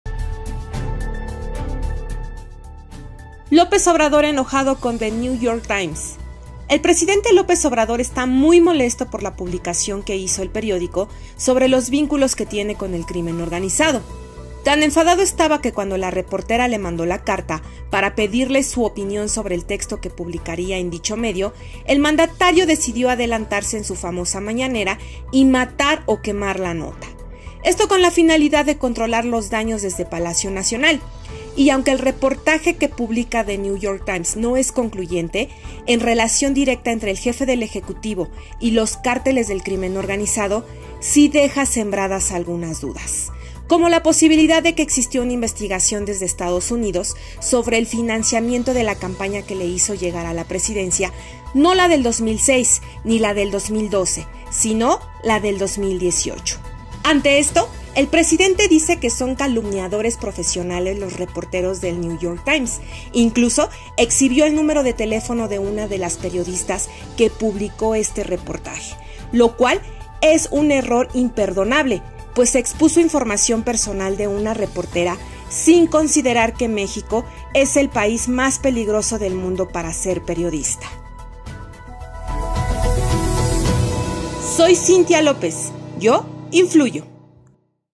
López Obrador enojado con The New York Times
El Inai dijo que iniciaría una investigación sobre la respuesta del presidente por una posible violación de una ley que protege los datos en manos de funcionarios del gobierno.